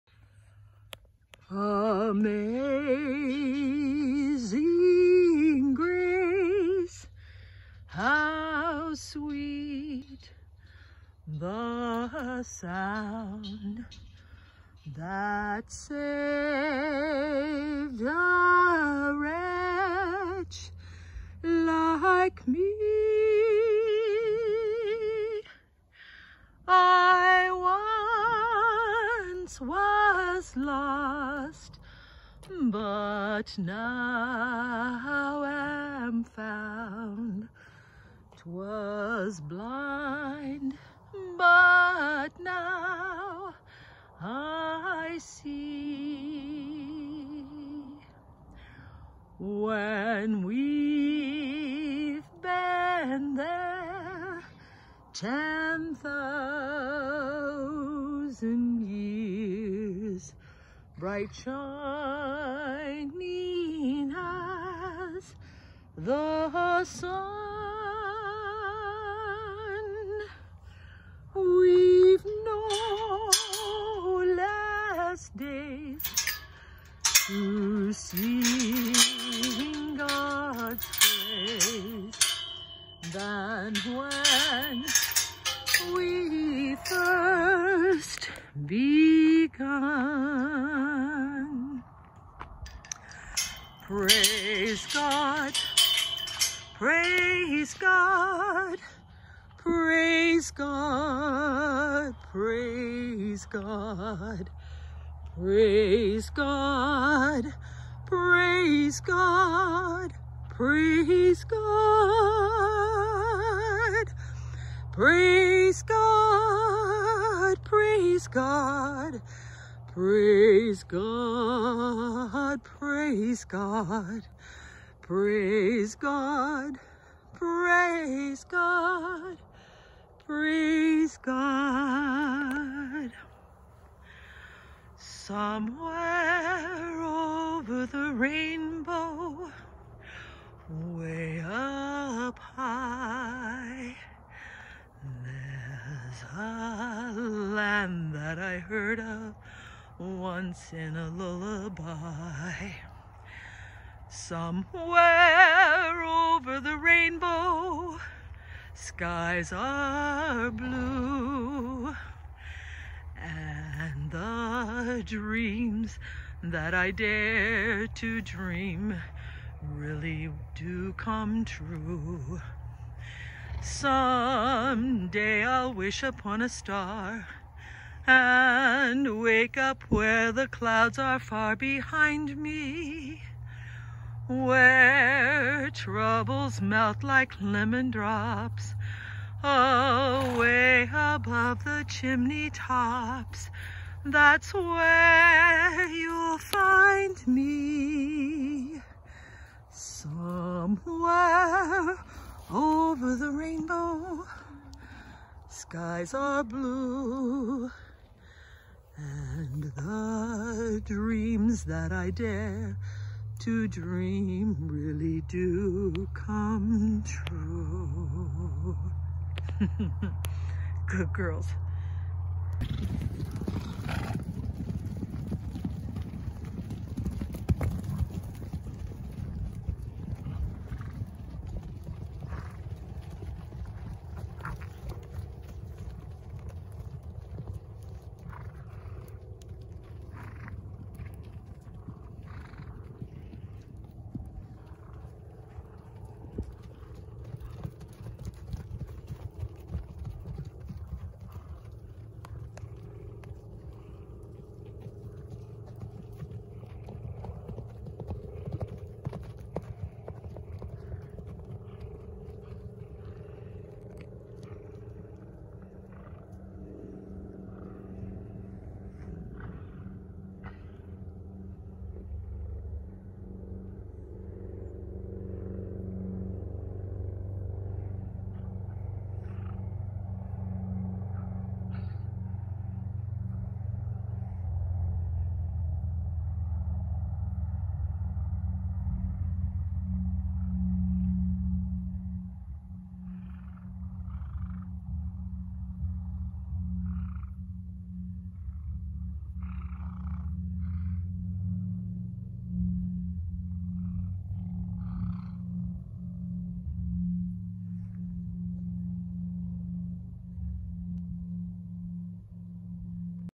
A little sing-song to the horses
I have been using a little singing when working one-on-one with our "wild-ish Mustang" Emanon,  He really seems to enjoy it and noticeably relaxes.

So this evening, on a whim, I decided to see how the horses might respond to my singing as I went out to let them out on pasture.

My voice is YEARS out of practice.  So please be gentle in your critique. 😁

Watch the horses closely.